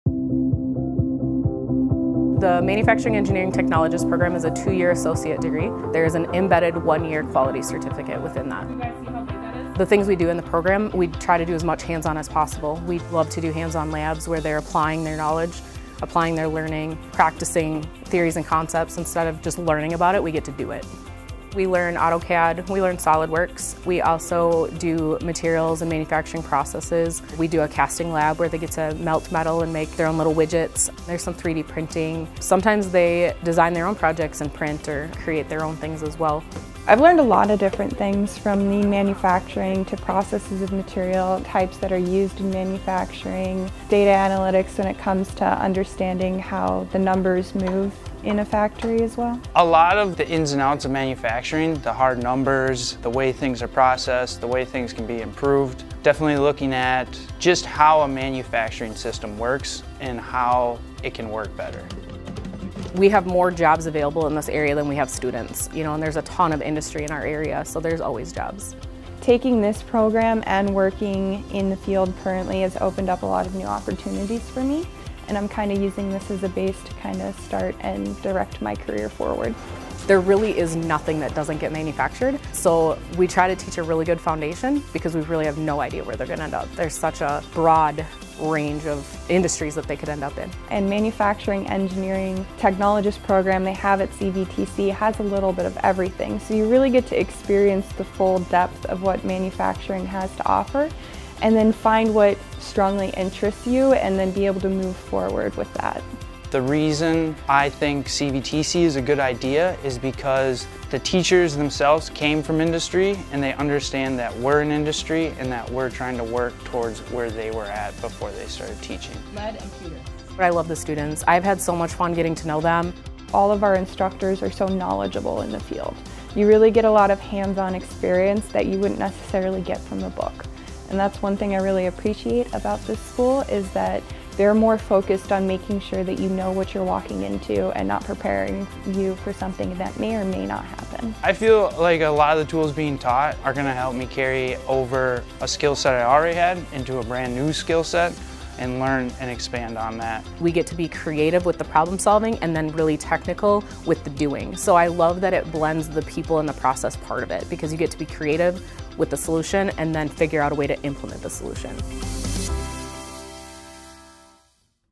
CVTC | Manufacturing Engineering Technologist Program
Use your science and math skills in the growing manufacturing sector. The Manufacturing Engineering Technologist program provides training for those seeking a career in engineering and engineering technology in support of metal fabrication, plastics processing, and consumer products manufacturing and assembly.

cvtc.edu/eeo